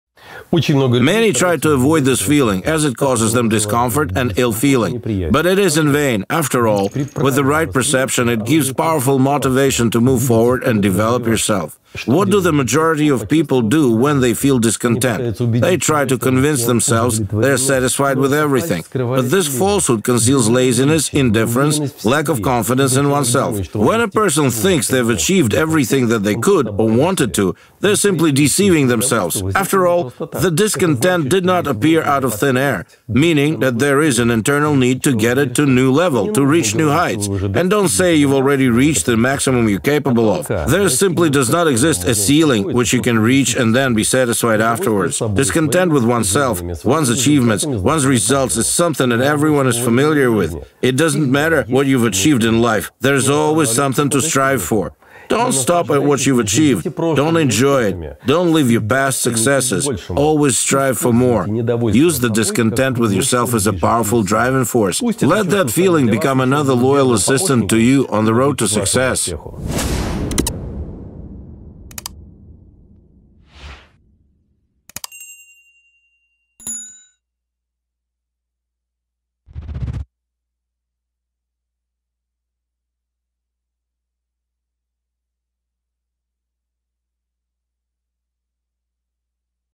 The secret to eternal motivation / Roman Vasilenko
It’s hard to overestimate the significance of motivation in achieving a goal. It’s the driving force that every person requires to overcome the difficult route to success, wealth and prosperity.

Therefore, you need to unceasingly maintain your motivation at a high level, then you’ll constantly find the strength in yourself to move further. The higher your motivation, the easier it is for you to cope with difficulties on the road to your goal.

However, motivation can decrease over time, and then appear the doubts and lack of self-confidence. People begin to give up and deny themselves their dream. Don’t let it reach such a state.

What should you do to maintain your internal fire forever? Use the effective method I’ve talked about in my video. Take action and you will succeed at everything!

Every day, I share insights and ideas with you on my instagram, follow the link and sign up, create your atmosphere for success

personal growth self improvement motivational speech morning motivation self discipline success inspirational personal development